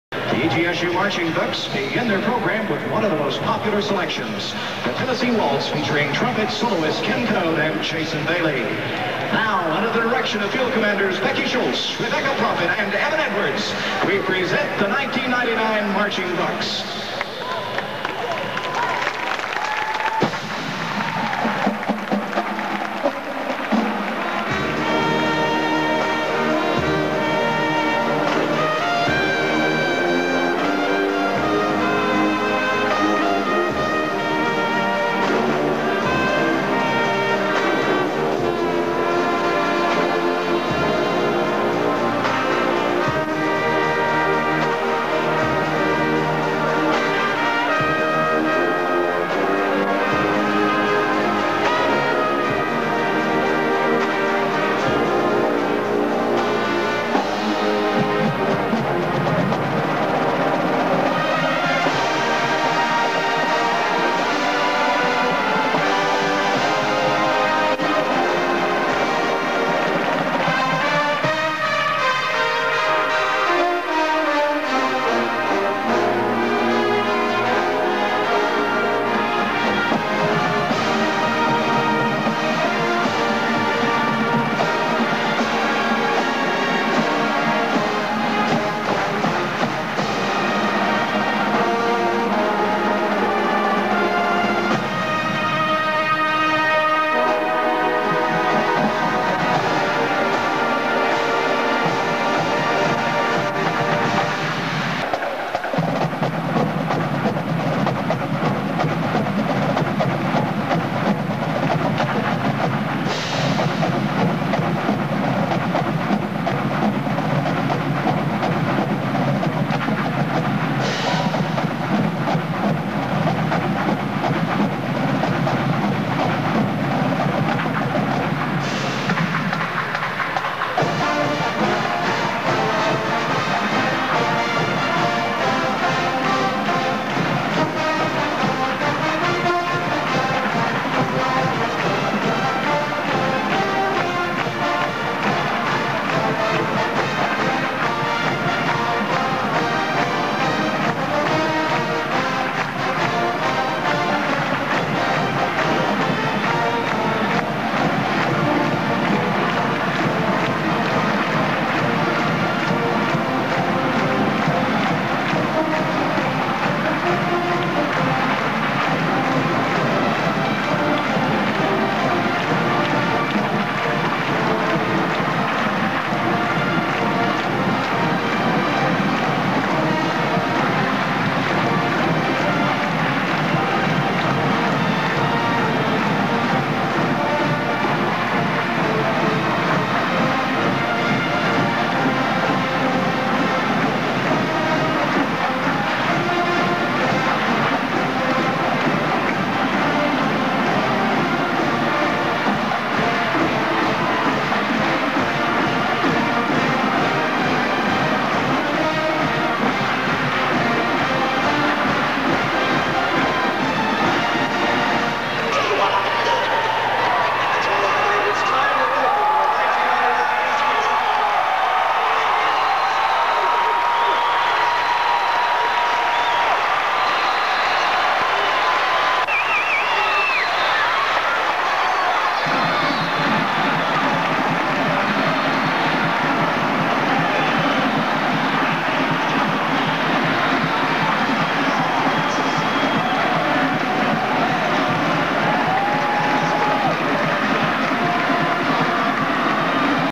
1999 East TN State University Marching Bucs Pregame
From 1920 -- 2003 East Tennessee State University in Johnson City, TN had a proud tradition of college football.

This is dedicated all participants, players, coaches, staff, band members, cheerleaders, dance squad, and fans.

Bring Back BUC Football.